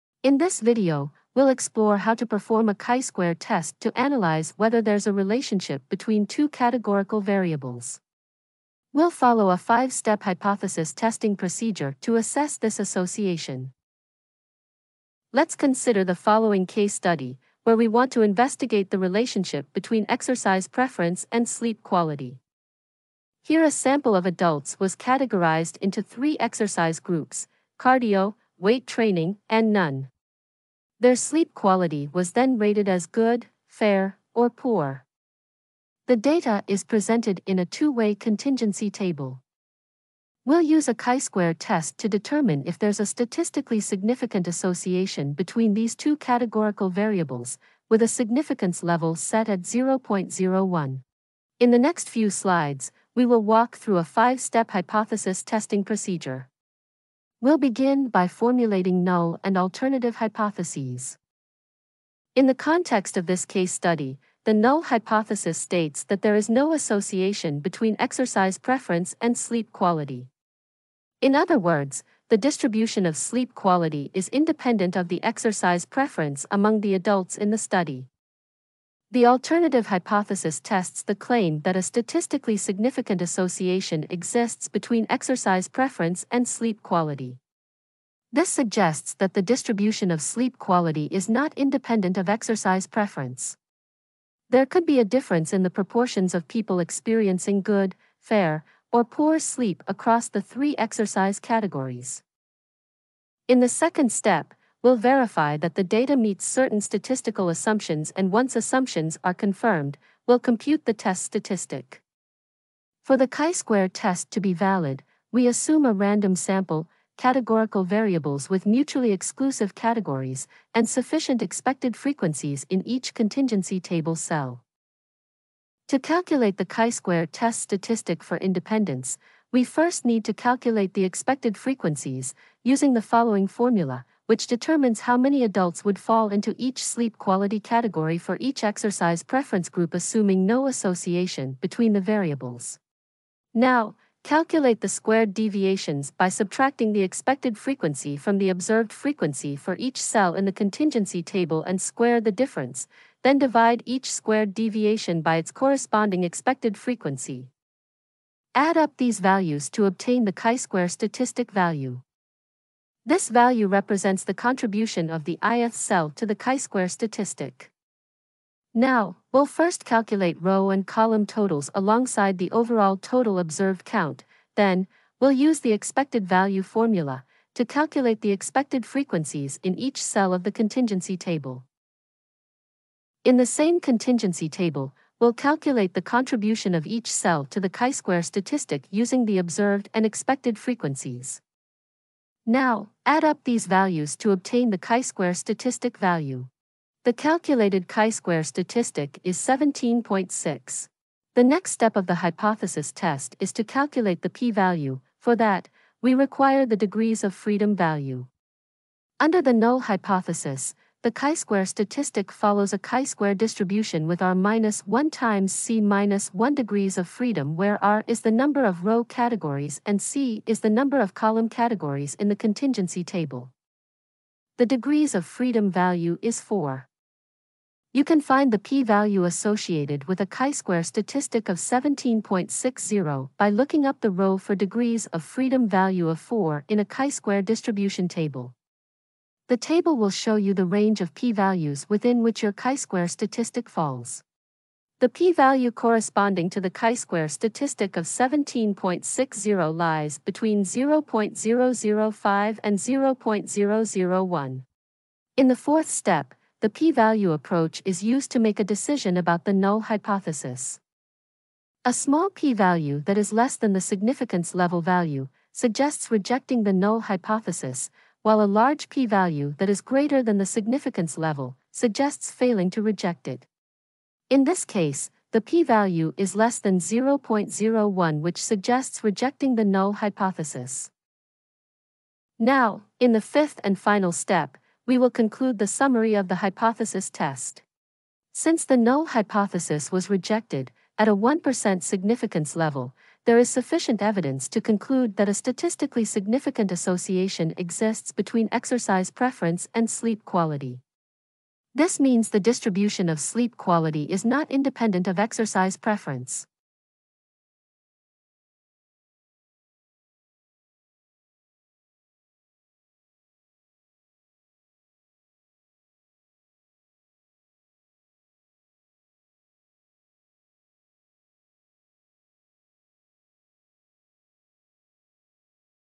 Chi-Square Test of Independence | A 5-Step Hypothesis Testing Guide
In this video, we'll use a chi-square test of independence to see if there's a connection between exercise preference (cardio, weight training, or none) and sleep quality (good, fair, or poor) in a sample of adults. We'll follow a step-by-step approach to explore this relationship and determine if it's statistically significant. By the end of this video, you'll understand how chi-square tests help us analyze relationships between categorical variables.